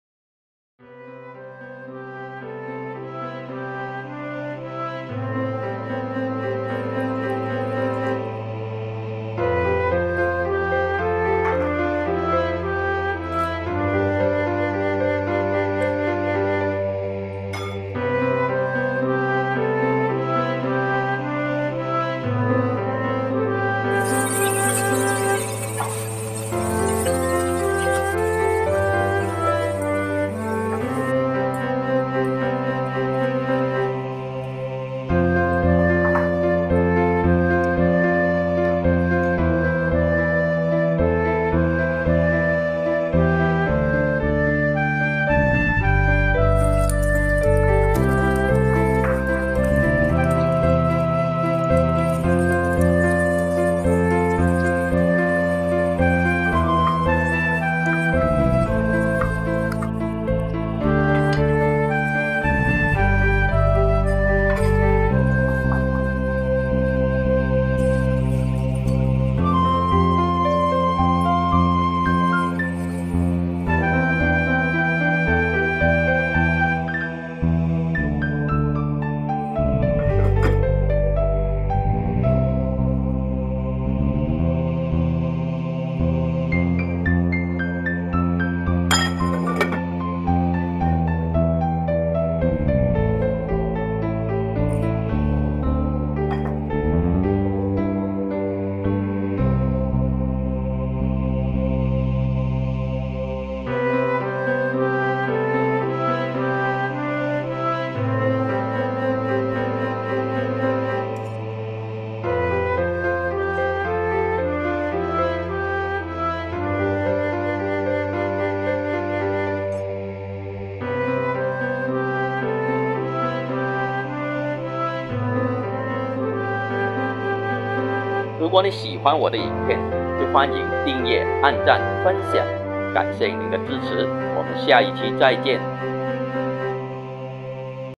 滷肉和滷蛋簡單做法分享
有煎過的五花肉就沒有那麼油膩了